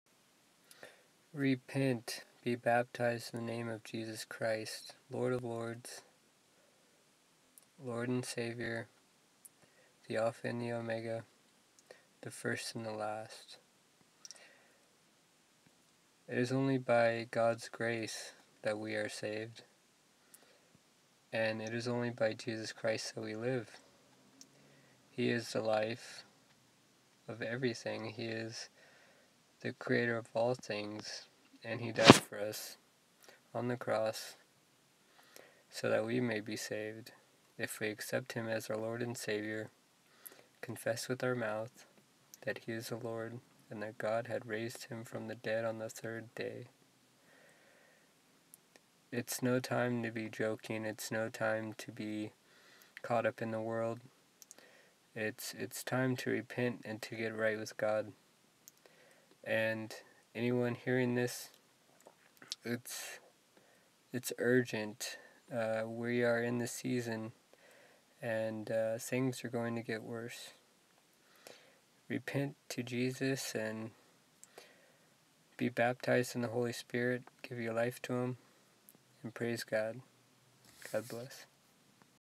Repent to Jesus
The times signs are all around, the end of days is near, repent and be baptized in the name of Jesus Christ, Lord of Lords, God embodied. Praise God!